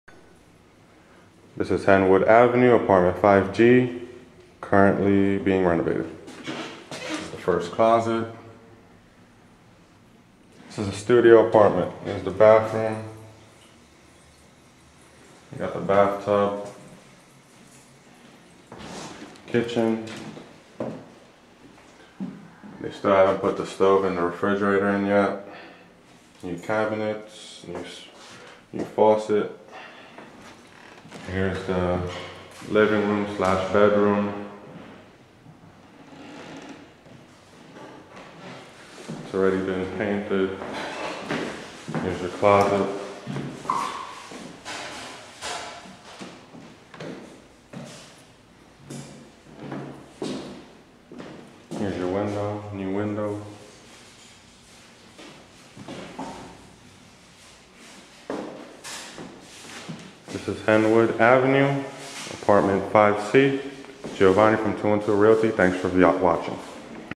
Henwood St unit 5C - Studio- $875/month
Studio apartment for rent in Mt. Hope, Bronx. Quiet area with local supermarket and convenience stores nearby. Close to Jerome ave and 4 Train.

G.Castro
Two One Two Realty
Licensed R.E. Salesperson
"Buying houses in Queens and Brooklyn"